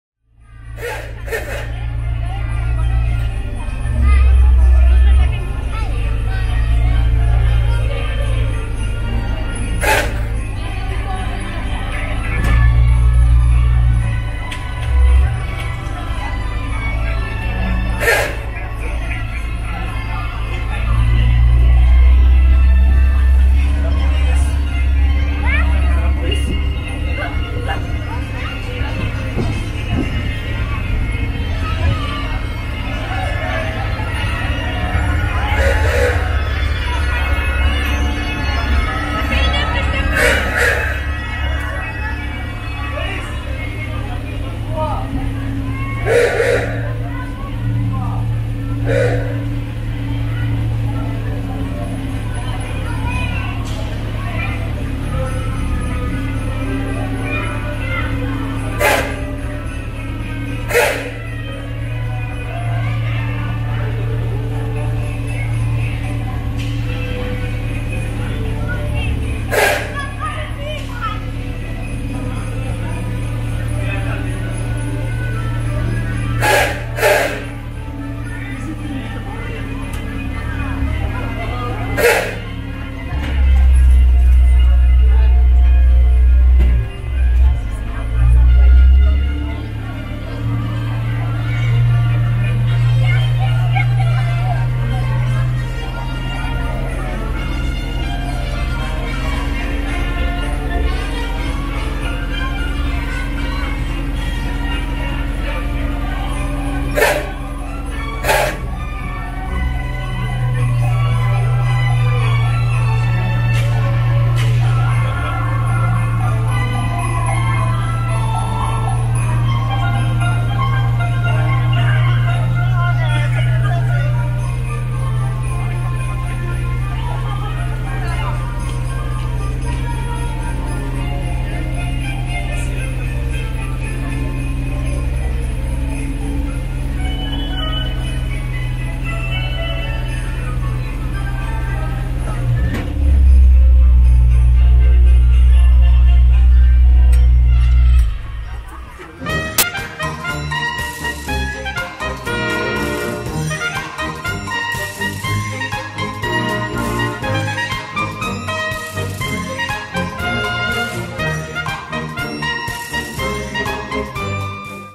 Christmas Drive Down Main Street USA - Christmas Disneyland Paris 2019 🎄
Christmas is the most magical season at Disneyland Paris, The perfect time to do a drive down Main Street USA  in one of the Main Street Vehicles.

Disney's Enchanted Christmas will officially run from November 9 2019 till January 6, 2020.

Be sure to check our other socials for live updates from the Disney Parks around the world. See ya real soon!